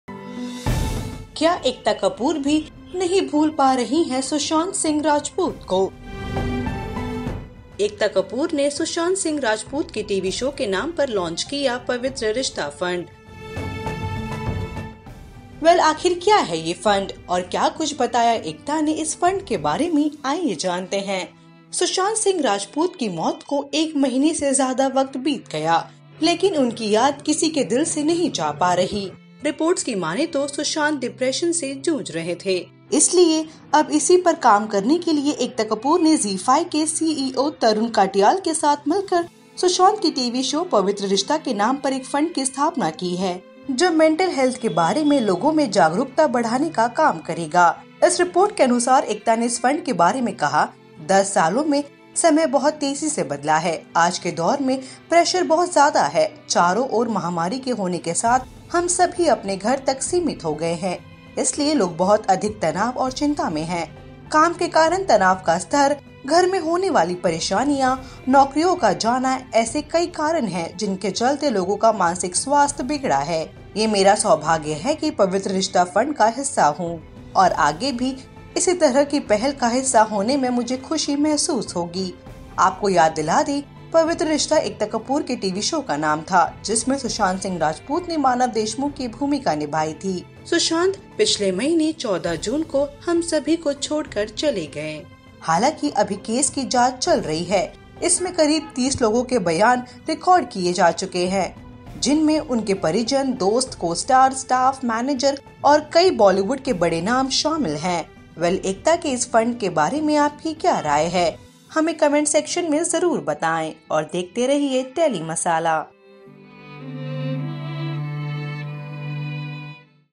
Ekta Kapoor Announces FUND For Mental Health Awareness In Memory Of Sushant Singh Rajput
Ekta Kapoor who had earlier launched Sushant Singh Rajput in her show Pavitra Rishta has announced a fund for mental health awareness in memory of the Actor. It has been named the Pavitra Rishta Fund.Watch Video To Know More.